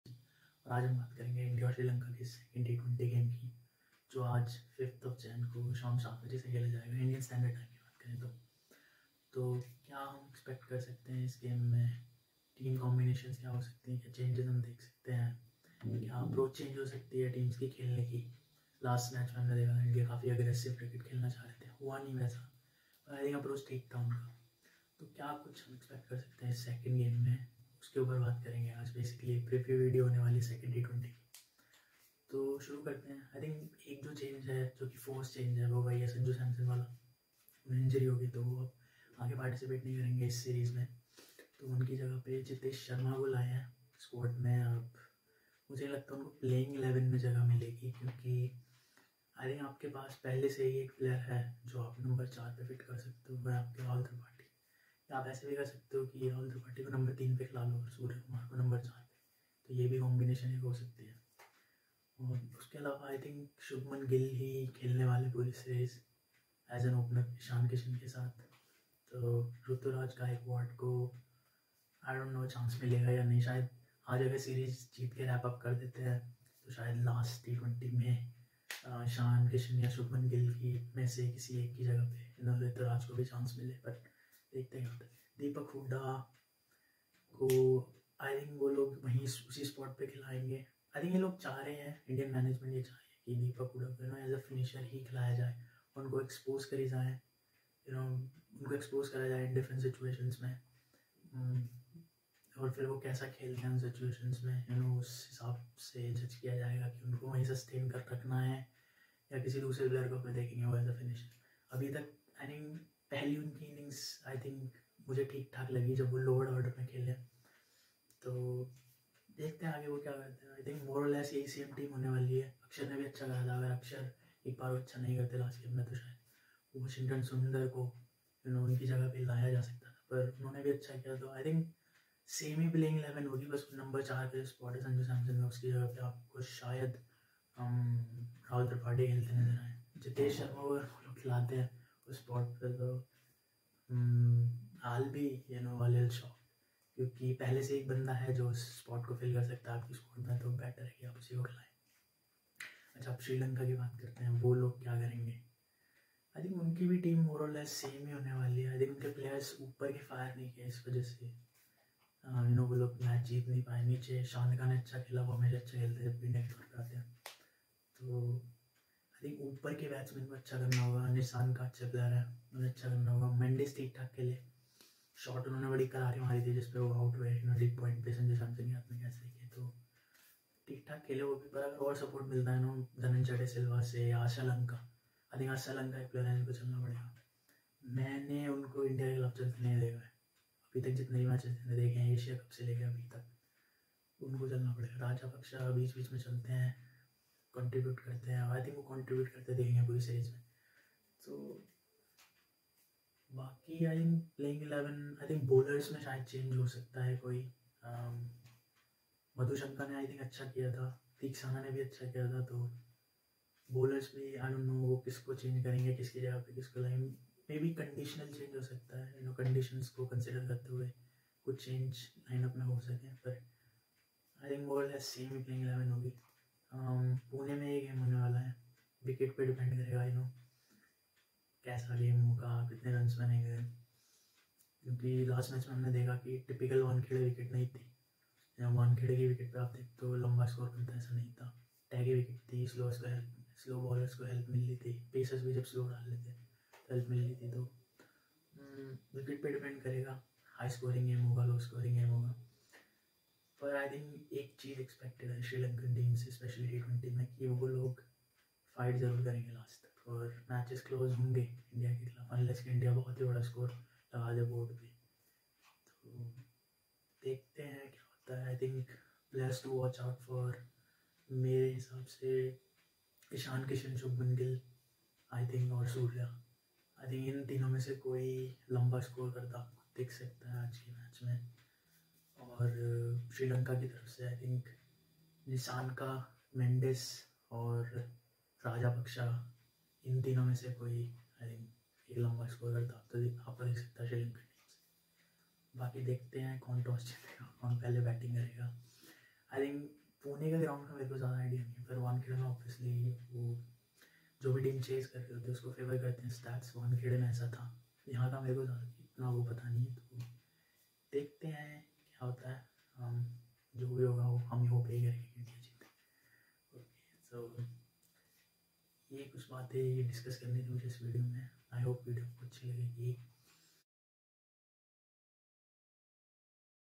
INDIA vs SRI LANKA 2ND T20I - Preview & Prediction
India takes on Sri Lanka in the second T20i of the 3 match series. Will Sri Lanka make a comeback or India will clinch this series by beating Sri Lanka the second time ??

Players who could fire in all cylinders today - top 3-4 of both teams.